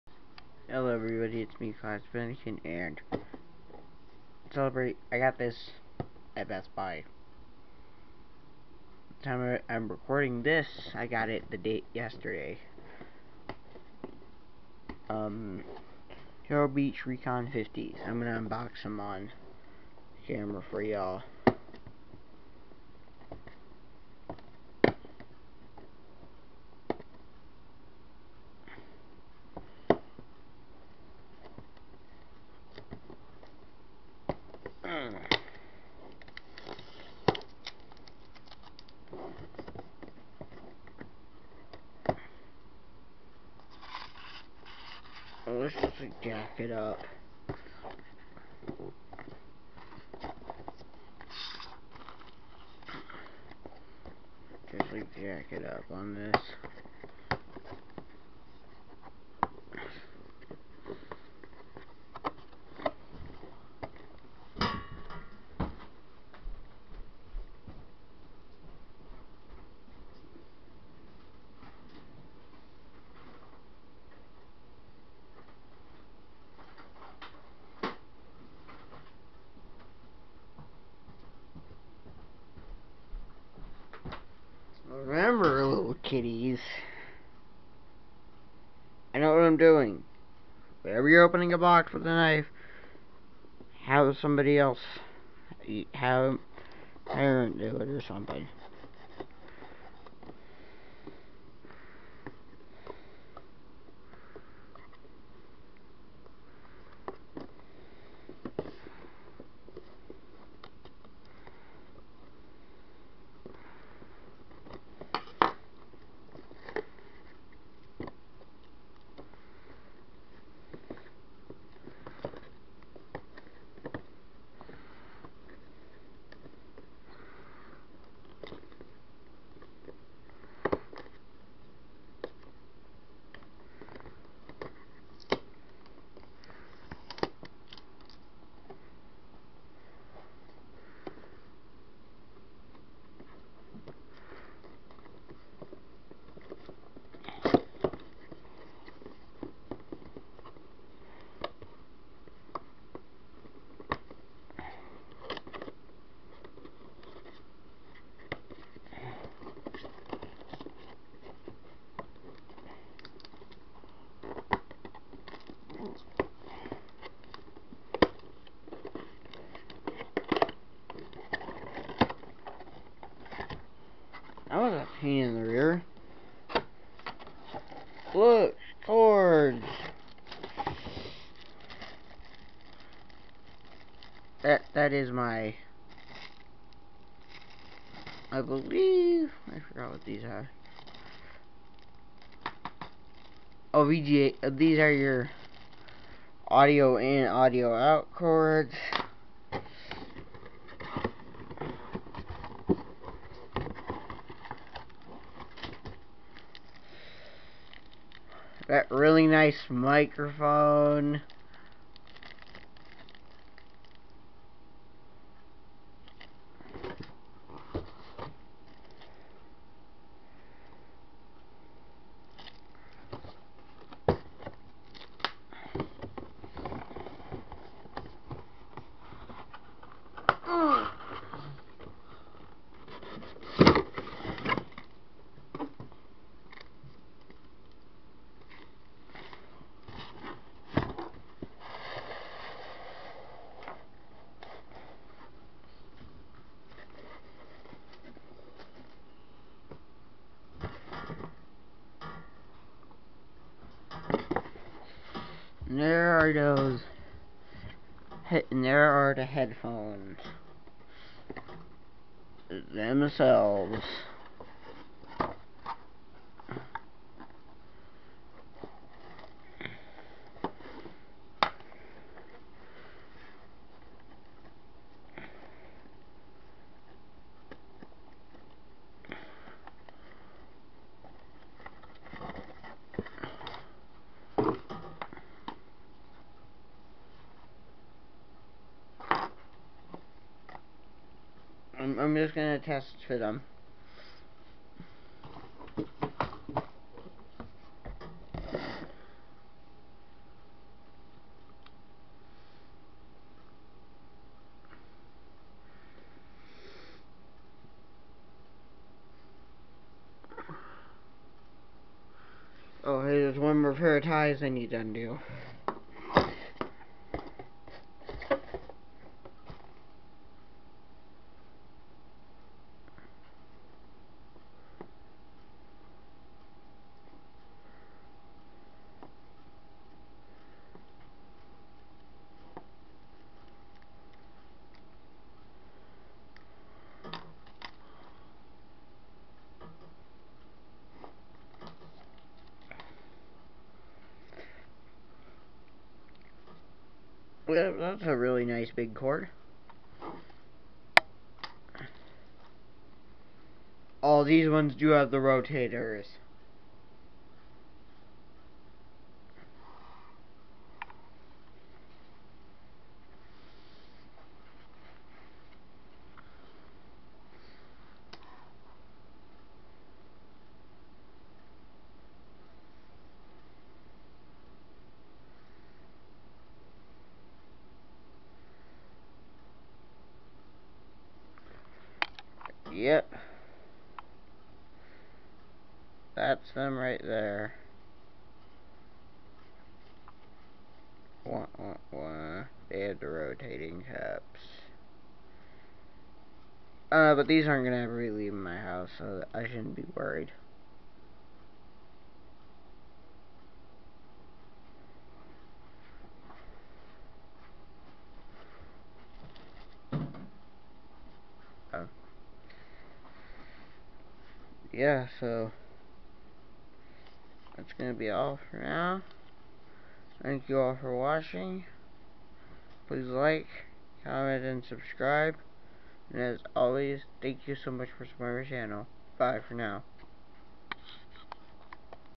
20 subs special- unboxing my turtle beach ear force recon 50s
Nintendo network id: classyfennekin

and also thank you all for getting to 20 subs, were now a a pretty good small channel